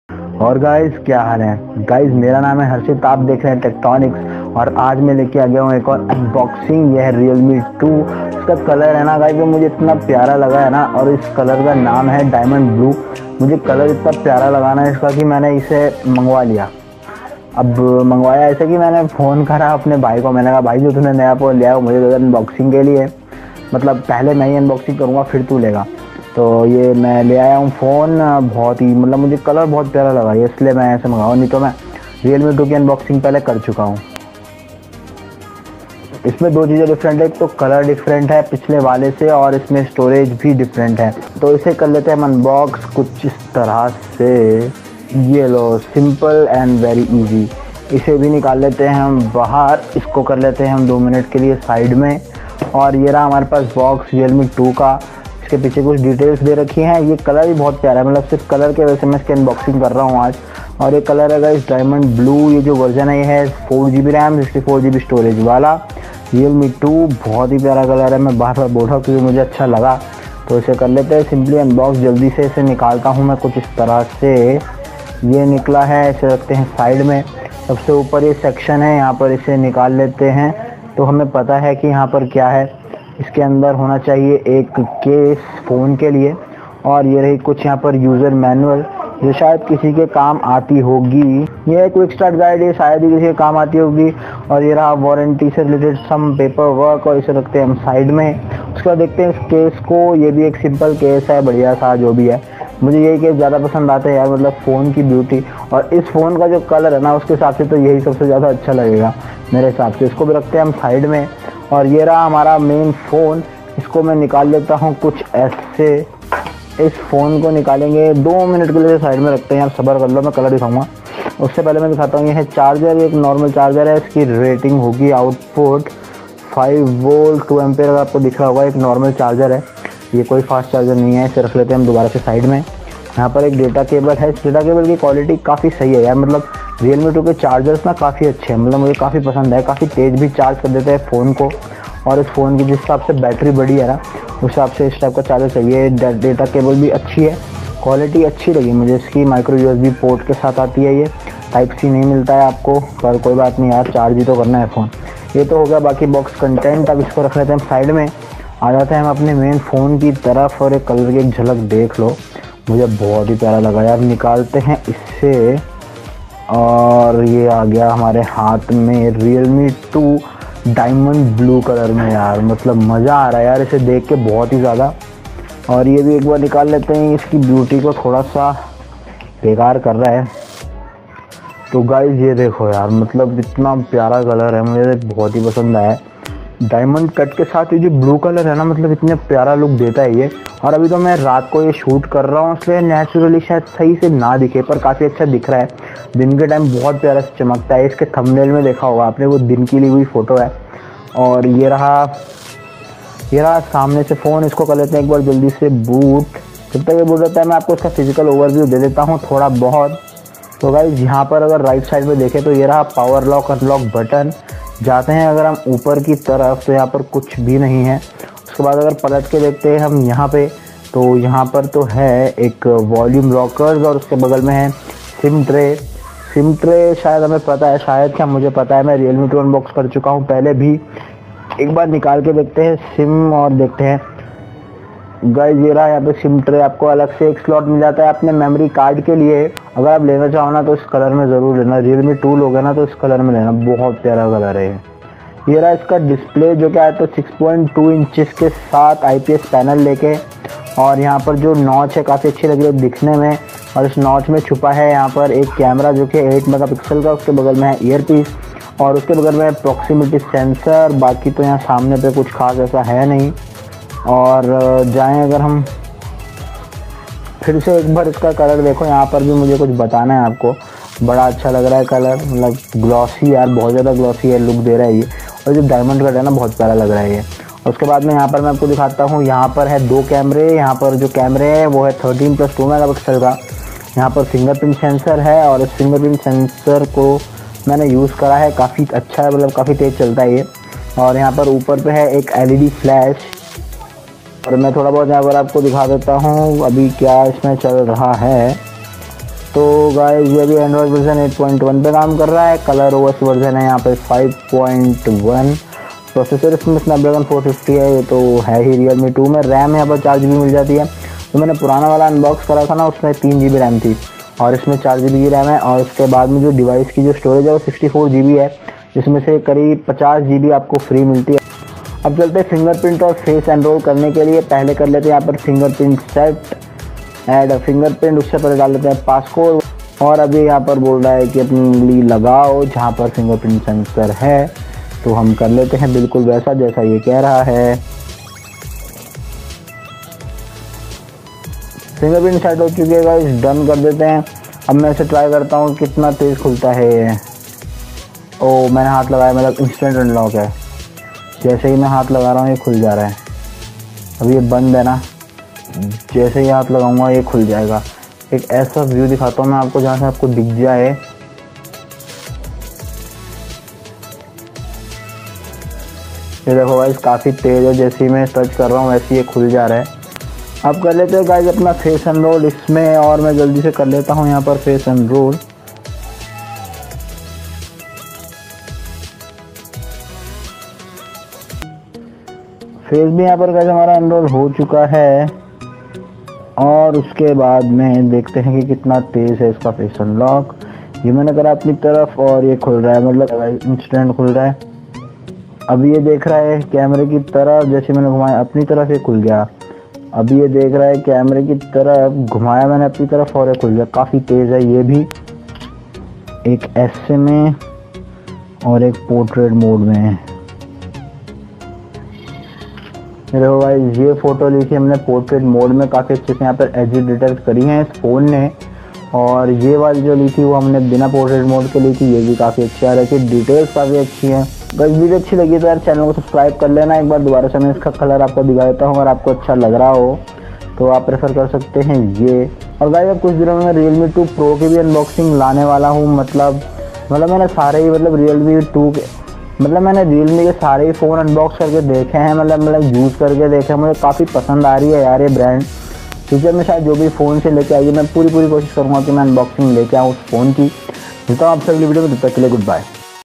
Realme 2 New Colour😍Diamond Blue🔥Looks Amazing 😍Unboxing |Tech Tonix
In this video i have unboxed Realme 2 Diamond Blue which is the new colour. It looks Super Cool..